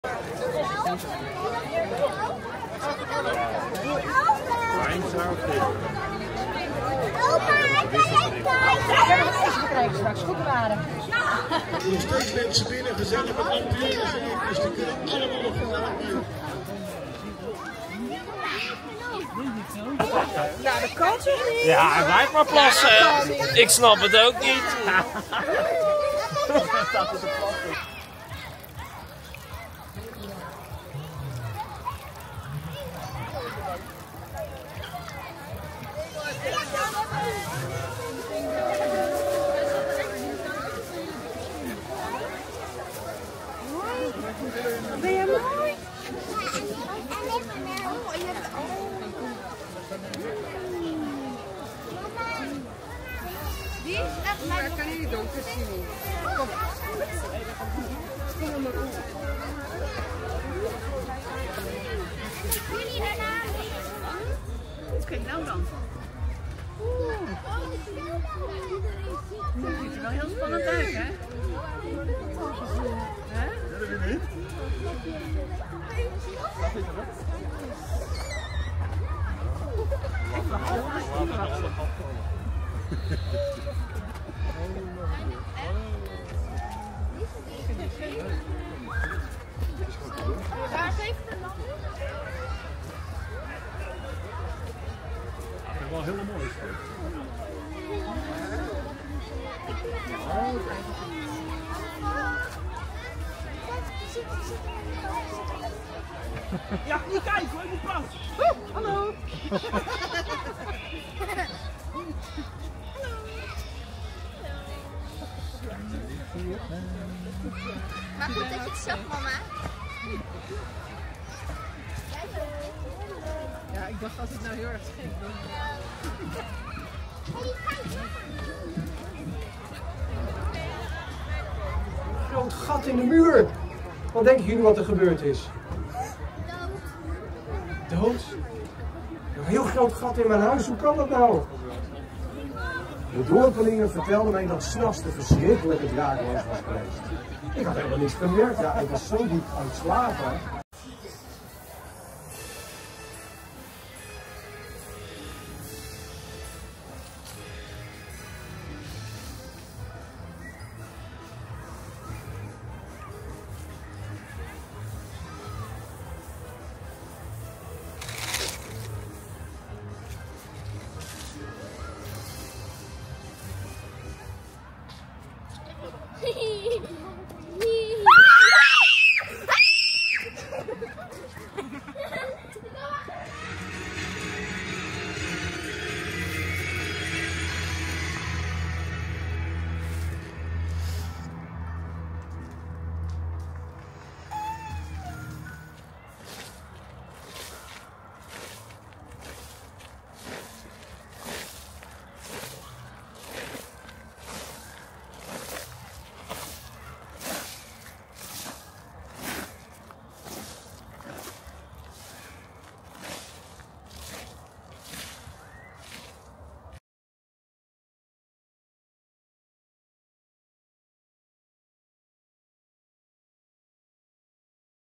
Halloween in Park Rusthoff
Het was weer griezelen in het Sassenheimse Park Rusthoff tijdens Halloween.